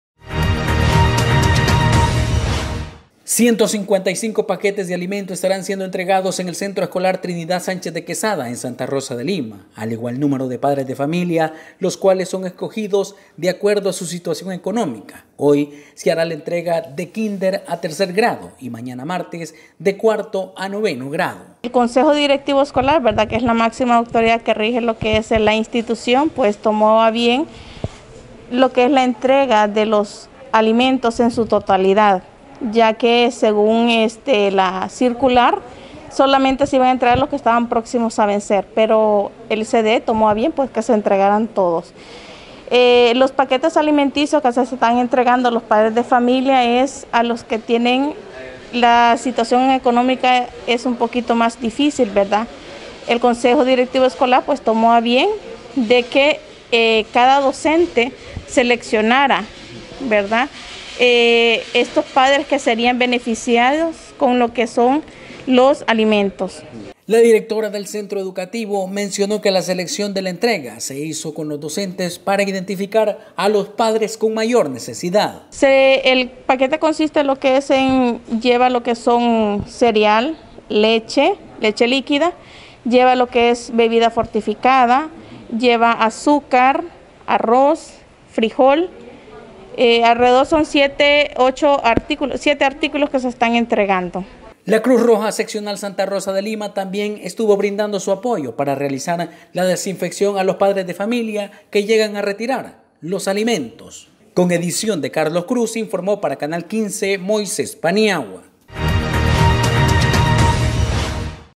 ENTREGAN PAQUETES DE ALIMENTOS EN EL CENTRO ESCOLAR TRINIDAD SÁNCHEZ DE QUEZADA EN S.R.L.
ENTREGAN PAQUETES DE ALIMENTOS EN EL CENTRO ESCOLAR TRINIDAD SÁNCHEZ DE QUEZADA EN SANTA ROSA DE LIMA